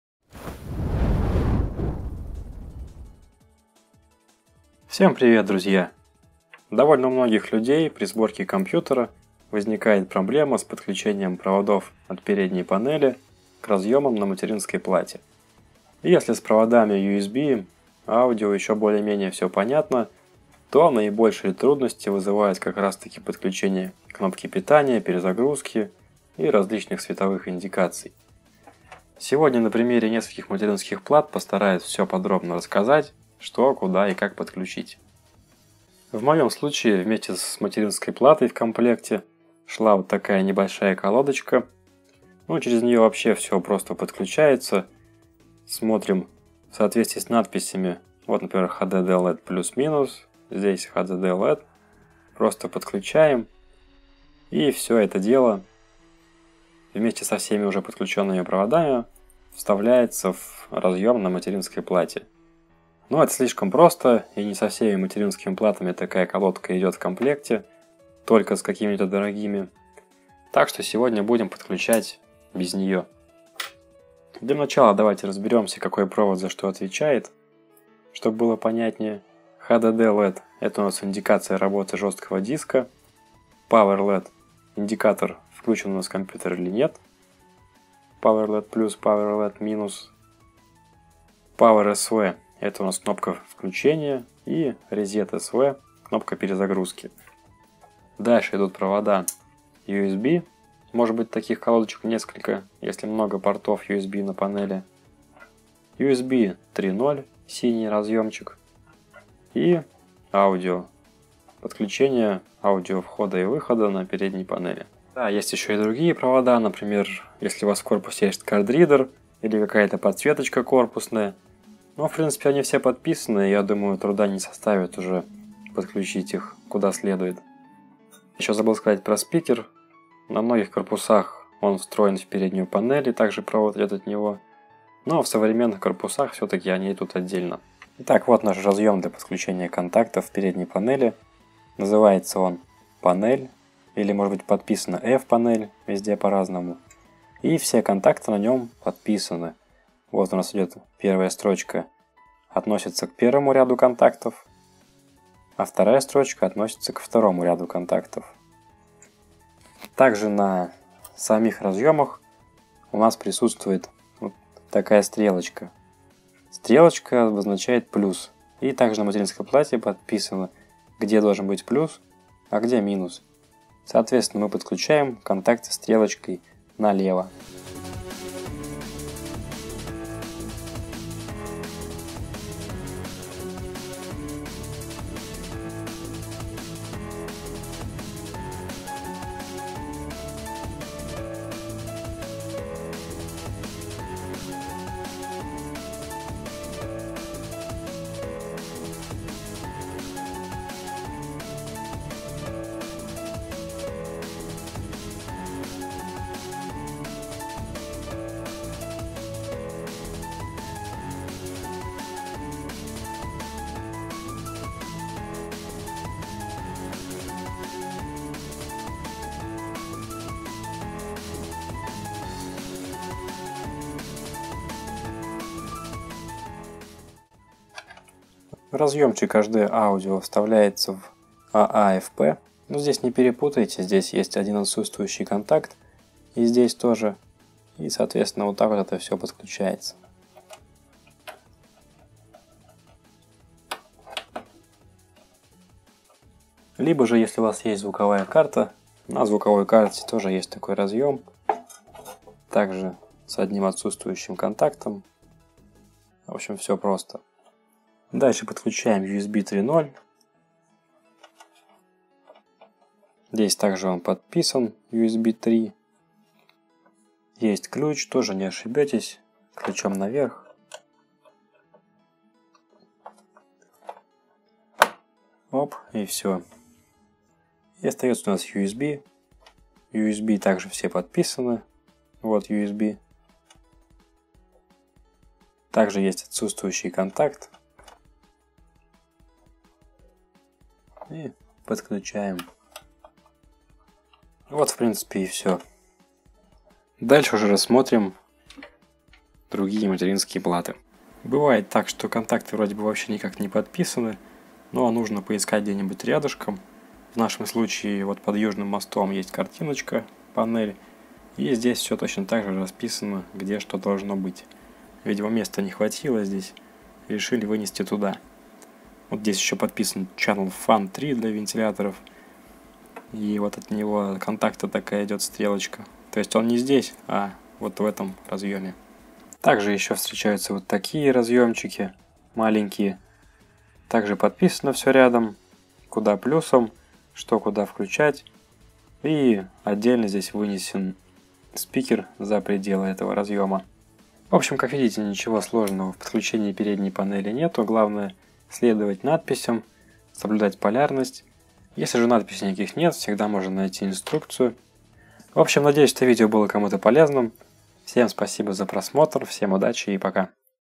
Как подключить переднюю панель компьютера к материнской плате?
Небольшая инструкция по подключению разъемов передней панели к материнской плате.

Содержание:
00:00 - 0:34 - Вступление
0:34 - 1:12 - Подключение через коннектор
1:13 - 2:17 - Какой провод для чего нужен
2:17 - 2:39 - Обозначения на разъеме "Panel" (fPanel)
2:39 - 3:00 - Какой стороной подключать контакты
3:00 - 3:47 - Подключение к "Panel" (fPanel)
3:47 - 5:00 - Подключение звука (HD audio)
5:00 - 5:44 - Подключение на примере старых материнских плат
5:44 - 6:01 - Заключение